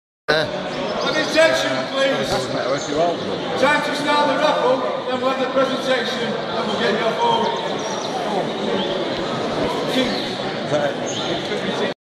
SHORT VIDEO OF MY MATE DOES WELL WITH HIS GREENFINCH
SHORT VIDEO OF MY FELLOW STEWARD AT SHOW DOES WELL WITH ALL HIS GREENFICH , ONE ON SHOW HIS GREENFINCH HEN .